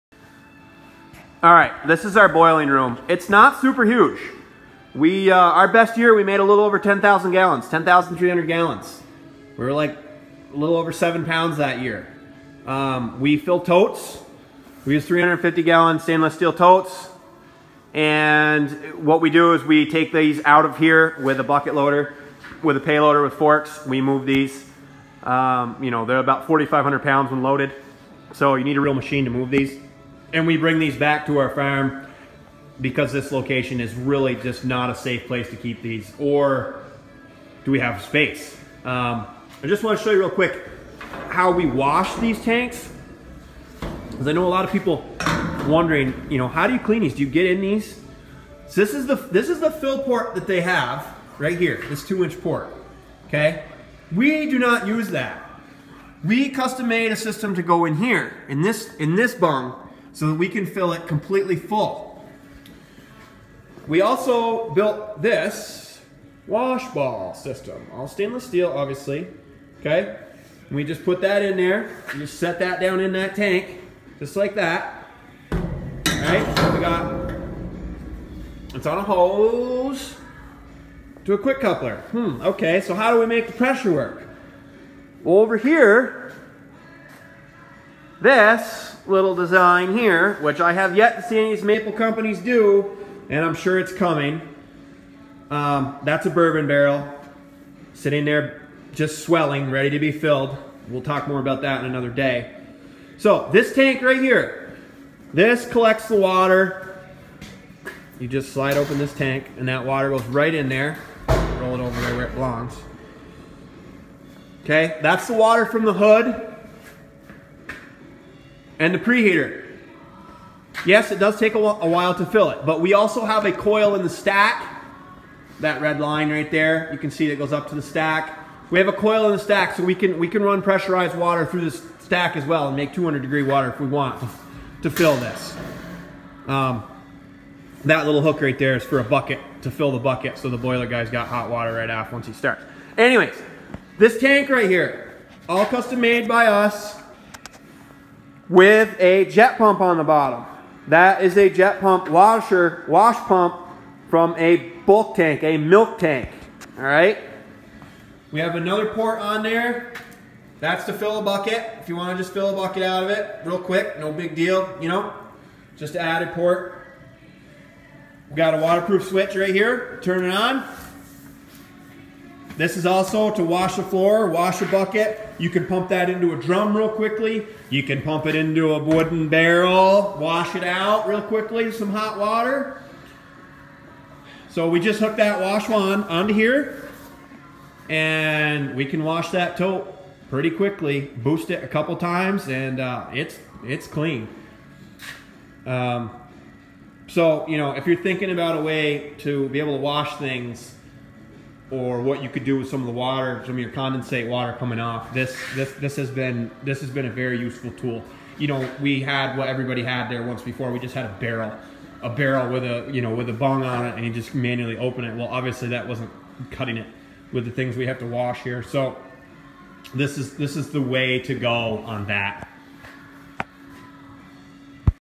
Evaporater condensate collection
This is a overview of our custom built condensation catch tank from our 6 x16 Evaporater